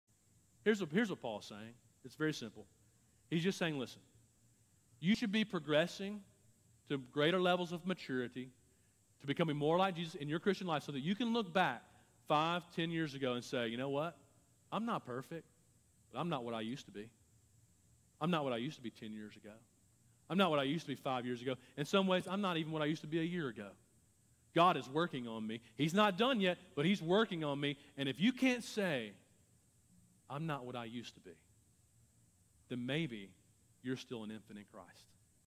Not What I Used to Be
As Christians, we not might be all we ought to be, but praise God we're not what we used to be!